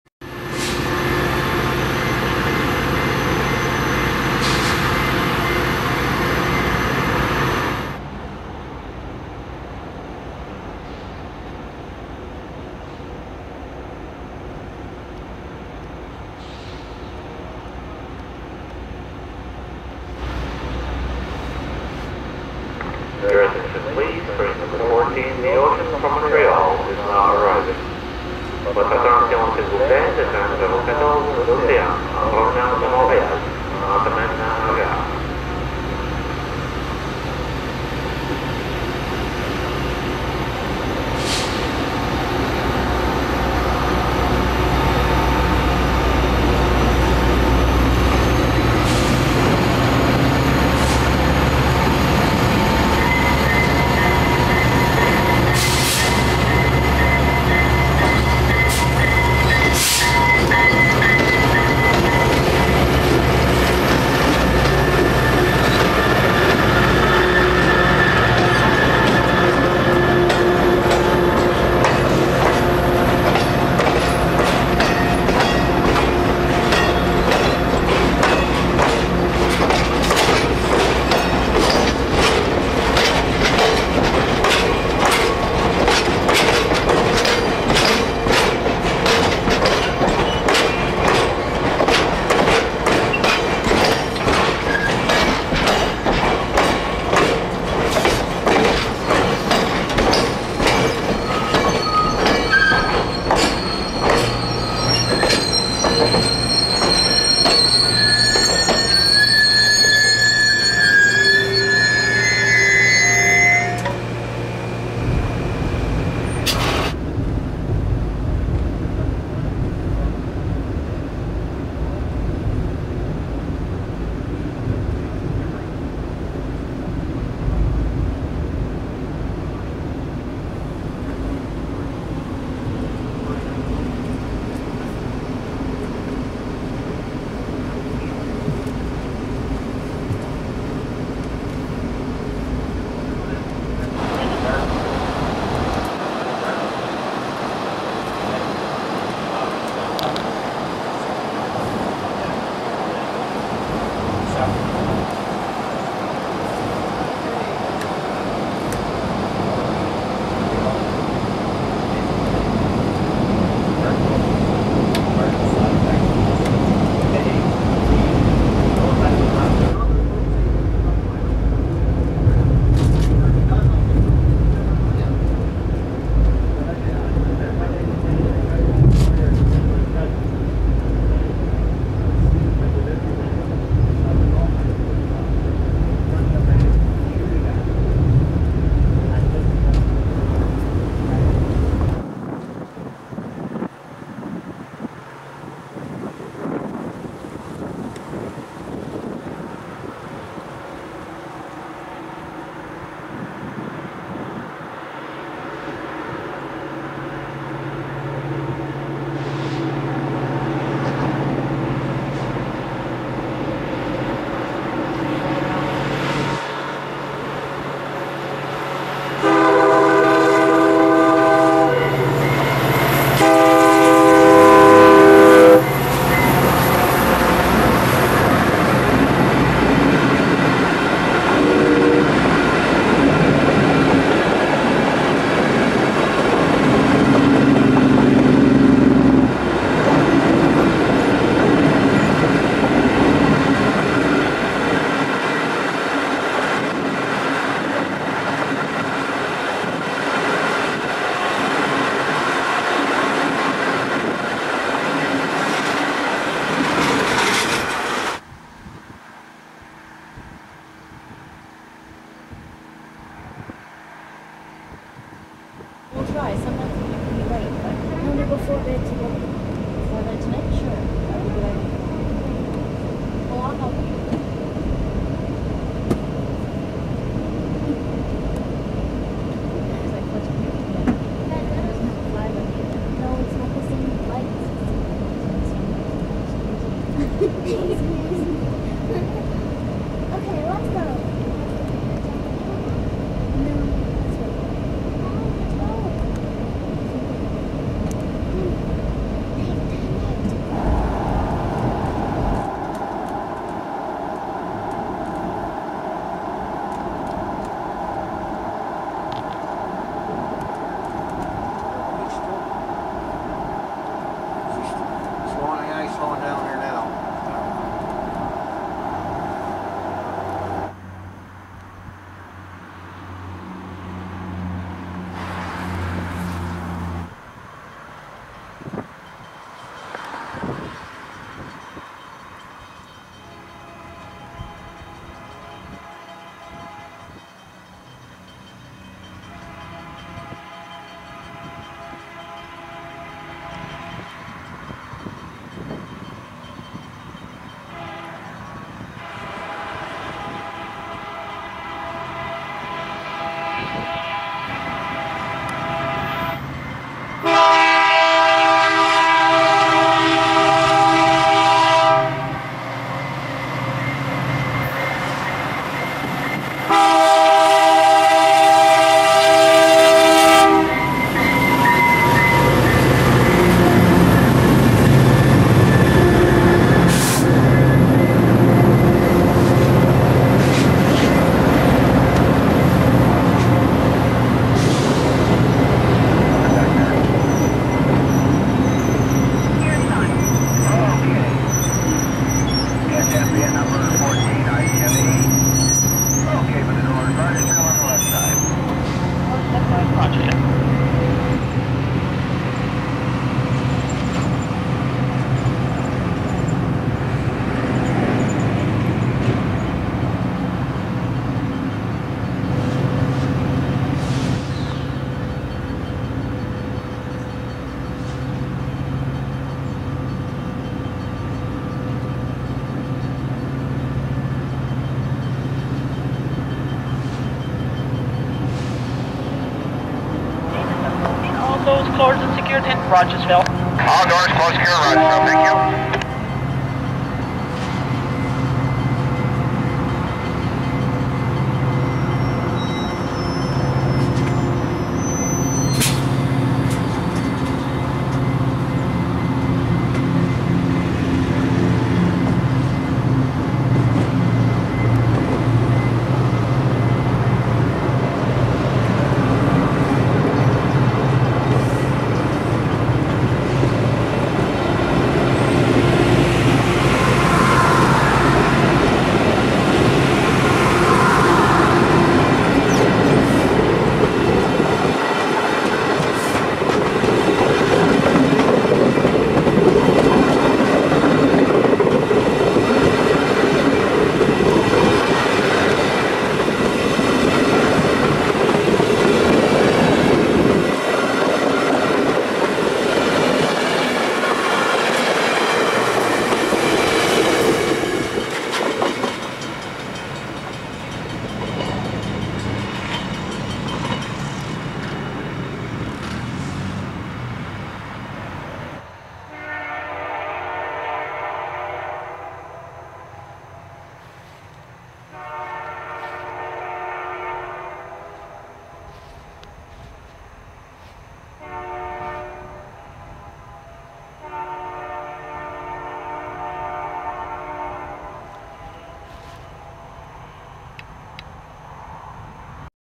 The VIA Rail "Ocean" Between Campbellton and Rogersville
This video is from the DVD "TRAINS in New Brunswick & Gaspé Volume 1 - Matapédia to Moncton".

This video features VIA Rail's "Ocean" train, with both trackside and onboard video, between Campbellton, NB and Rogersville, NB. The train is shown at the Campbellton, Miramichi and Rogersville stations as well as points in between.

This video was uploaded by Steve Boyko after David's death, on the request of David's widow Pat. She wants to share David's work with everyone. Apologies for any errors or omissions in the captions.

This video was recorded using a camcorder on VHS videotape and then digitized, so the quality is not as good as David's later work.